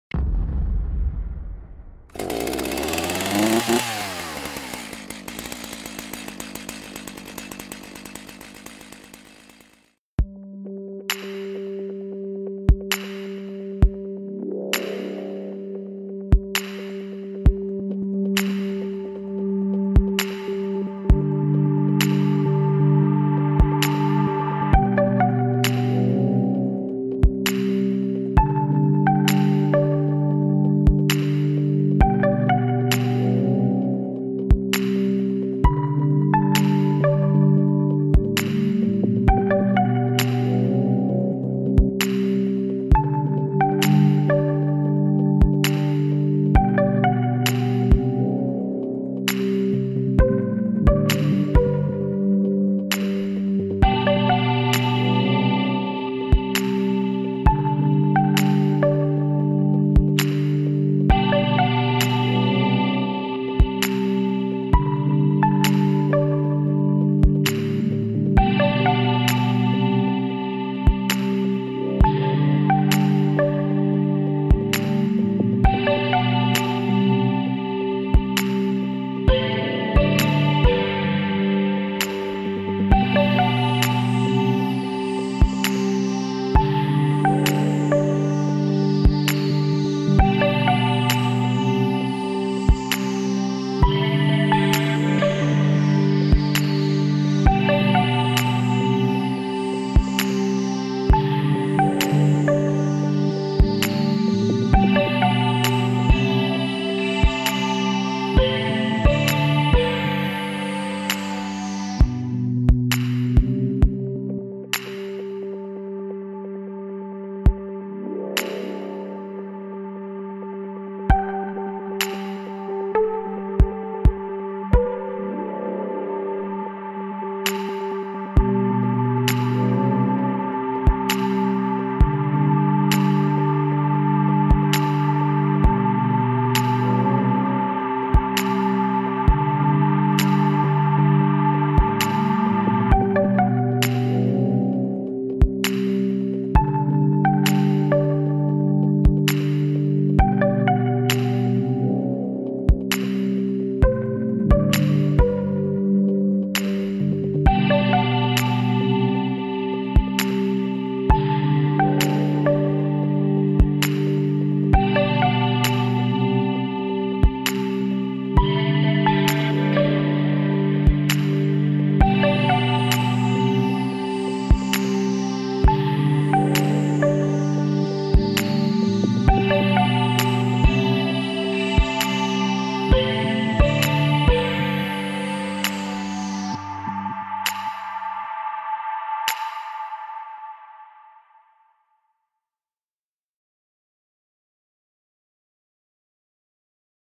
Chainsaw Carving a Triceratops - Chainsaw Dave
Chainsaw Dave: Time-Lapse Chainsaw Sculpture - 'Triceratops'

Watch as Chainsaw Dave creates a dramatic Triceratops sculpture from a huge lump of Oak. This was a commission for Horsham Council to go into a new Dino Island Play Park at Southwater country park, England.